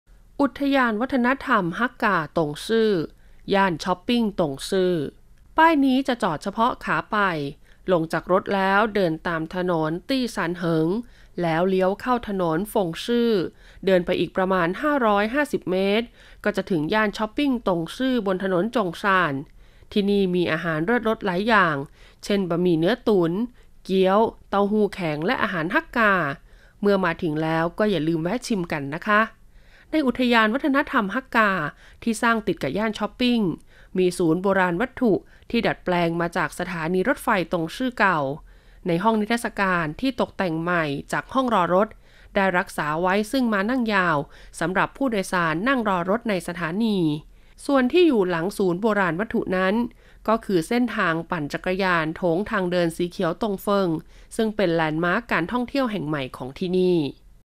【D6】สวนสาธารณะหลี่หยูปั๋วกง
อุทยานวัฒนธรรมฮากกาตงซื่อ
ย่านช็อปปิ้งตงซื่อ

ป้ายนี้จะจอดเฉพาะขาไป ลงจากรถแล้วเดินตามถนนตี้ซานเหิง และเลี้ยวเข้าถนนเฟิงซื่อ เดินไปอีกประมาณ 550 เมตร ก็จะถึงย่านช็อปปิ้งตงซื่อ (ถนนจงซาน) ที่นี่มีอาหารรสเลิศหลายอย่าง เช่น บะหมี่เนื้อตุ๋น เกี๊ยว เต้าหู้แข็ง และอาหารฮากกา เมื่อมาถึงแล้วอย่าลืมแวะชิมกันนะคะ ในอุทยานวัฒนธรรมฮากกาที่สร้างติดกับย่านช็อปปิ้ง มีศูนย์โบราณวัตถุที่ดัดแปลงขึ้นมาจากสถานีรถไฟตงซื่อเก่า ในห้องนิทรรศการที่ตกแต่งใหม่จากห้องรอรถ ได้รักษาไว้ซึ่งม้านั่งยาวสำหรับผู้โดยสารนั่งรอรถในสถานีไว้ ส่วนที่อยู่หลังศูนย์โบราณวัตถุนั้น ก็คือเส้นทางปั่นจักรยานโถงทางเดินสีเขียวตงเฟิง ซึ่งเป็นแลนด์มาร์คการท่องเที่ยวแห่งใหม่ของที่นี่